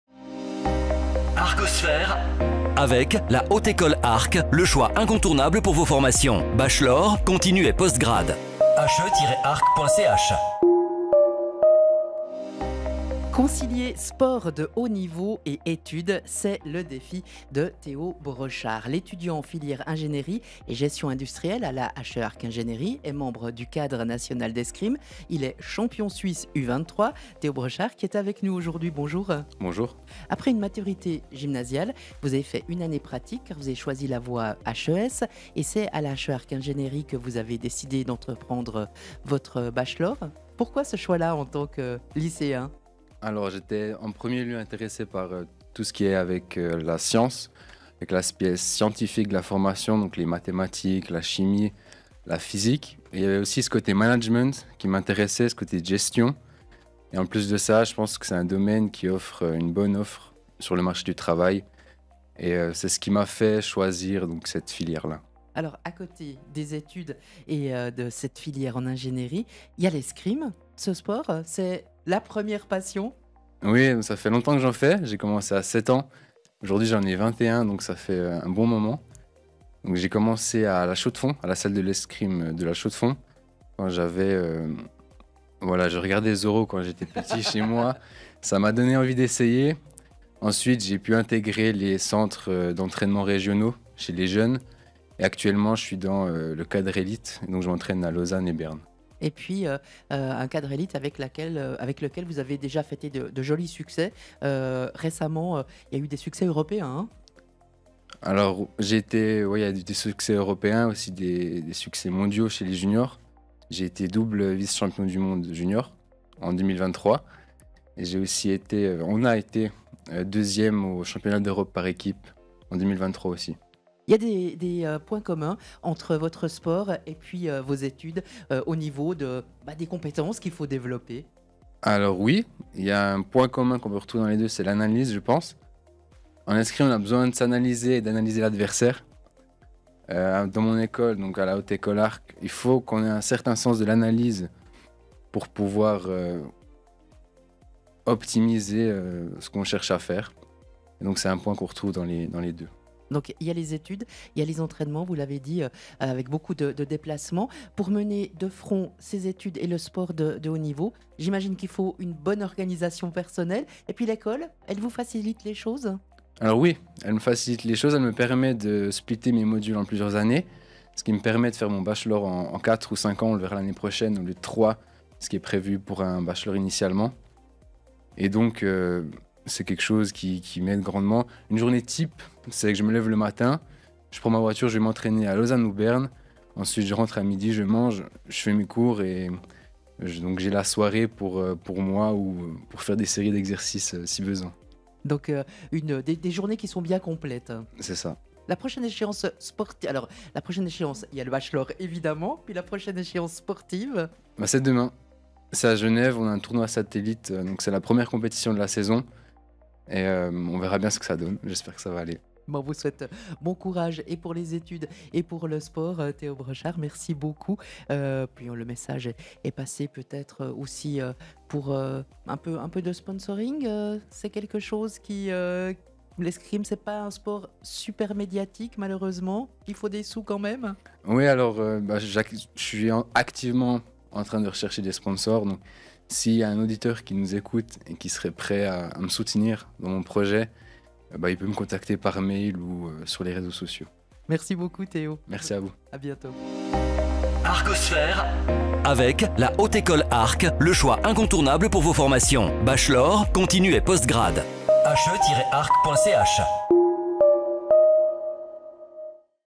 ARCOSPHERE | Théo Brochard - Concilier sport de haut niveau et études en haute école
Concilier sport de haut niveau et études en haute école tel est le défi de Théo Brochard, étudiant en filière Ingénierie et gestion industrielle à la HE-Arc Ingénierie et membre du cadre national d’escrime. On parle de cette organisation bien spécifique avec lui.